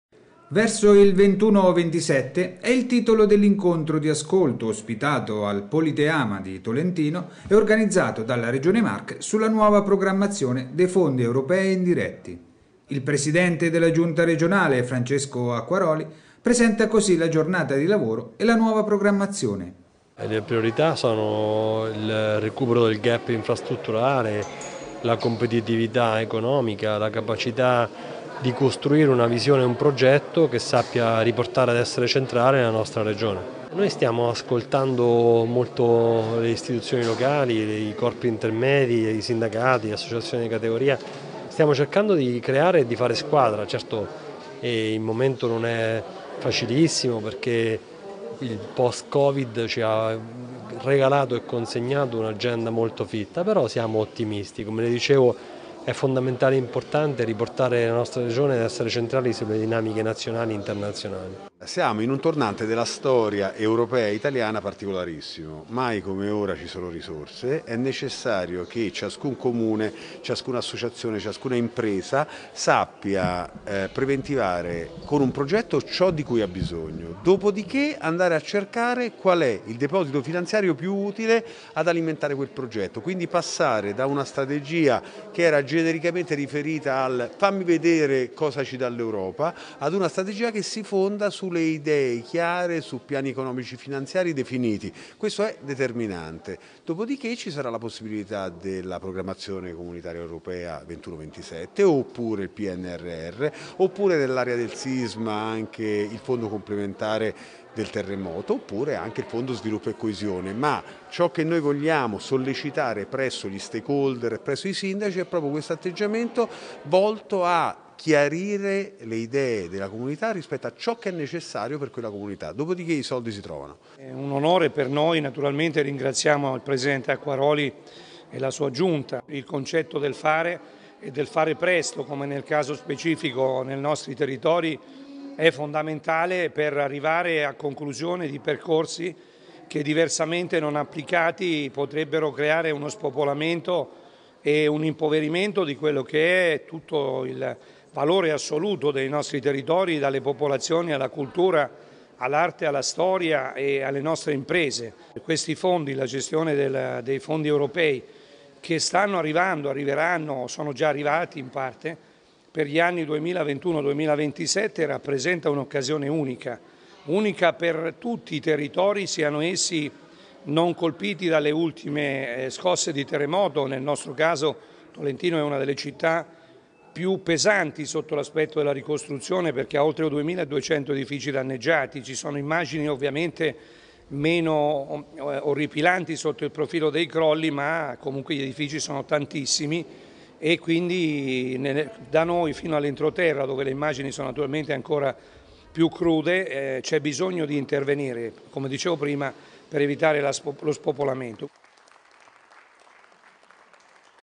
Evento di ascolto della Regione Marche per programmazione fondi europei a Tolentino-Video Tolentino
Il servizio di VideoTolentino realizzato venerdì 22 ottobre a Tolentino in occasione dell'incontro della Regione Marche con le istituzioni provinciali in vista della nuova programmazione.